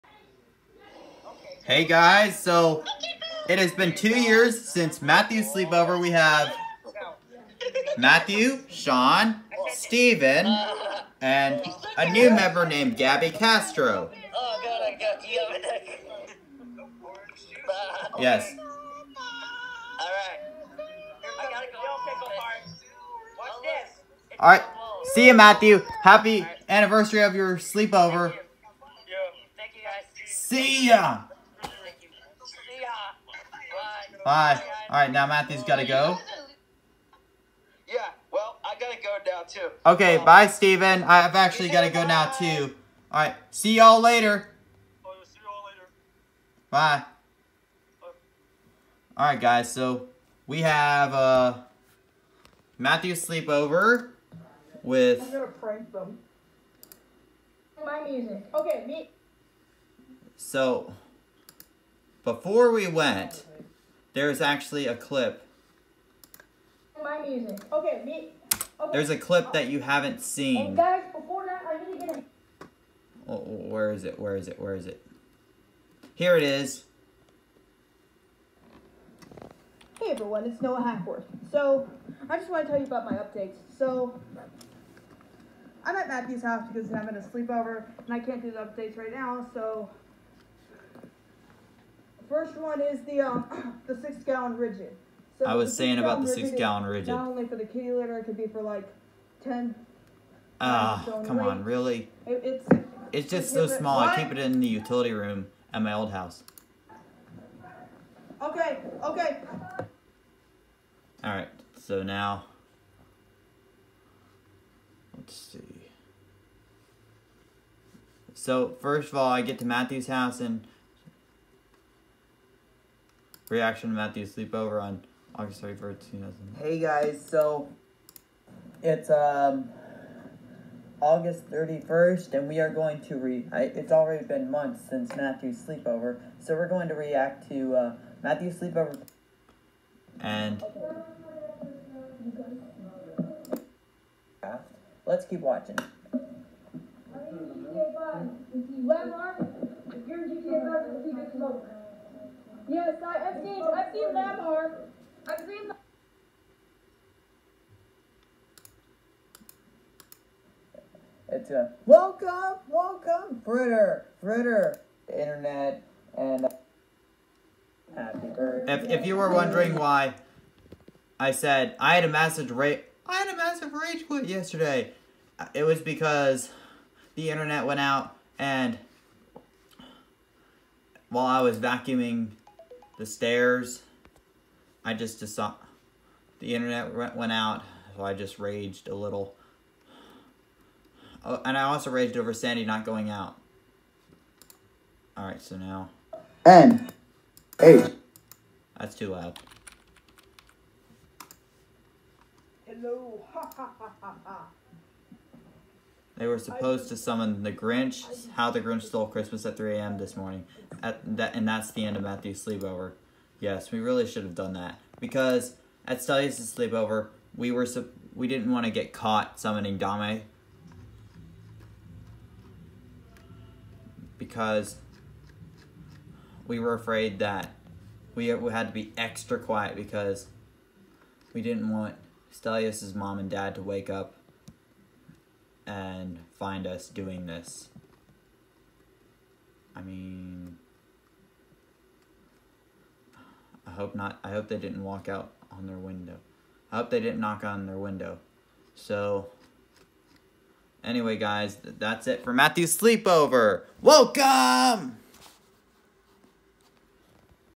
HAPPY 2 YEAR ANNIVERSARY MATTHEWS SLEEPOVER!!!!!!!!
Two years ago today on December 8, 2018 me Matthew Stephen and Stelios had a sleep over at Matthew’s house! So two years later I’m reacting to the videos that I made there!